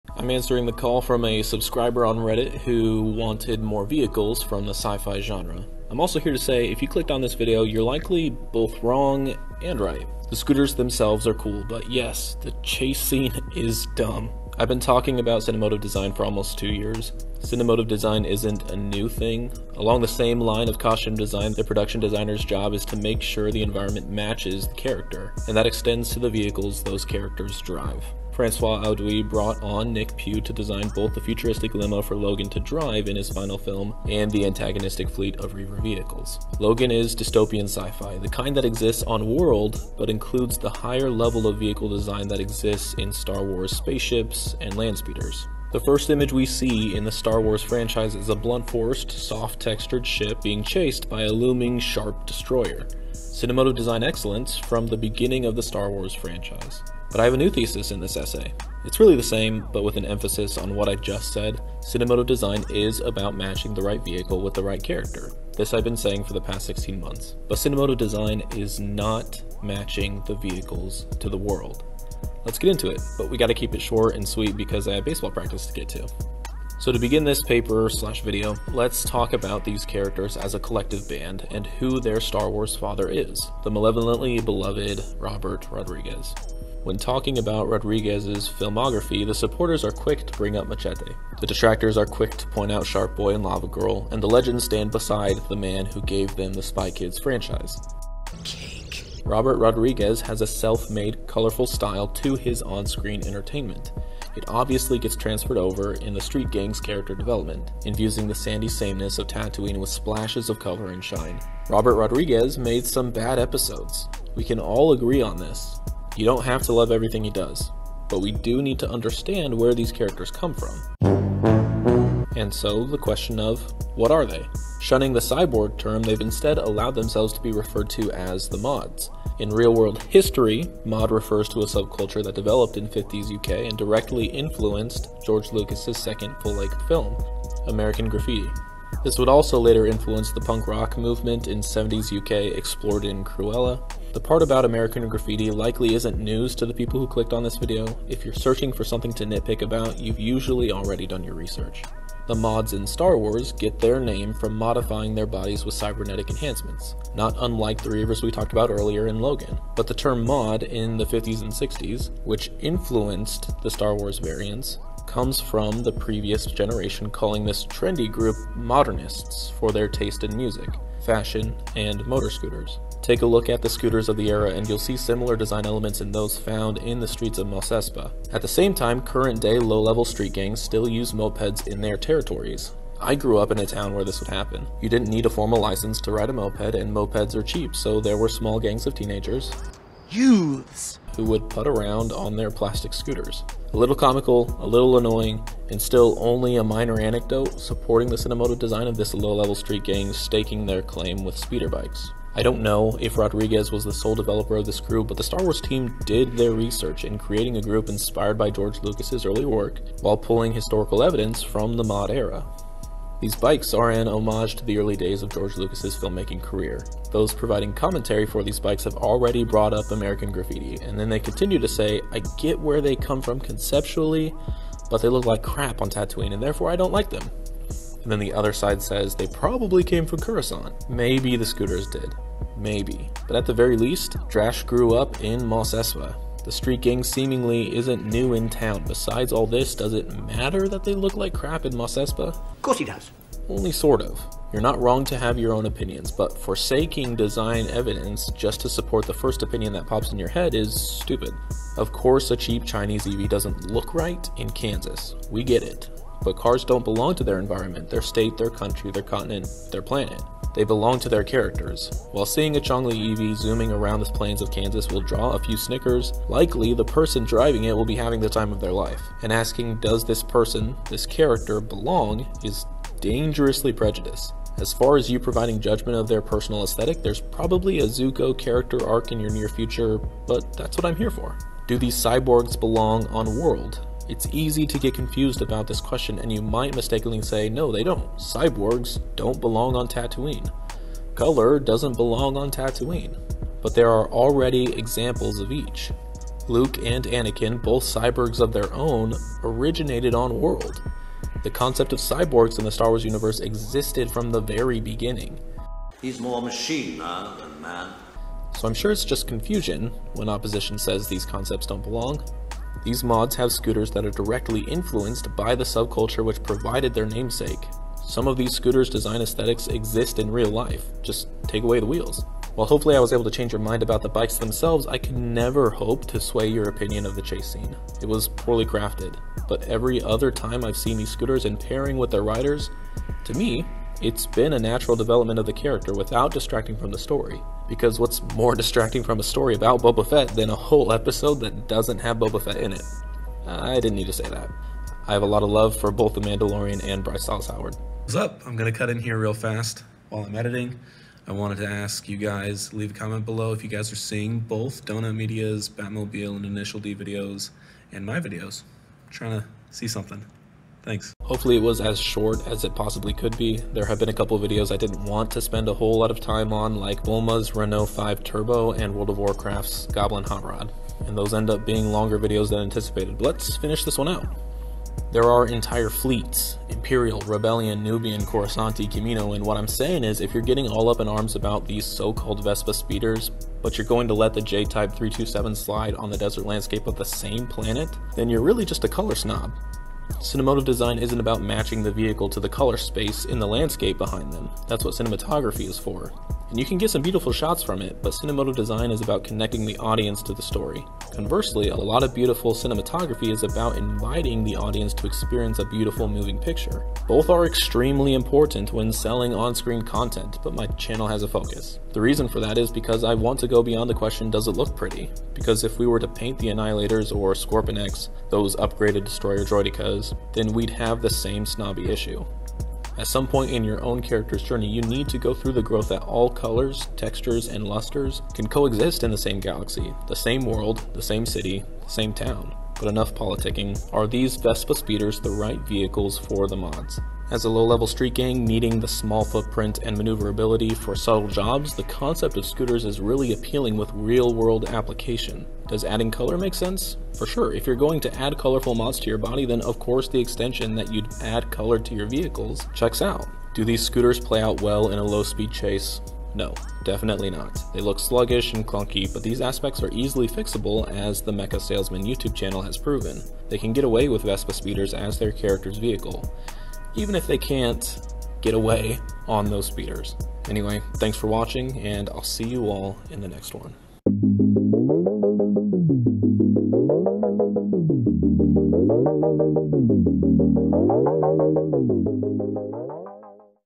Why These Bikes Are Better Than You Think | Mos Espa Scooters
Boba Fett's gang of cybernetic Mods have been made fun of for their Power Ranger bikes. Laugh away at the low speed chase, because that was terrible. BUT! Those bikes are actually kinda perfect for them.

Watch the videos linked below! Anyway, I hope you enjoy this video! JW

0:00 - Welcome & Introduction
1:16 - The Gang of Mods
3:33 - Mos Espa Speeder Bikes
6:15 - Conclusion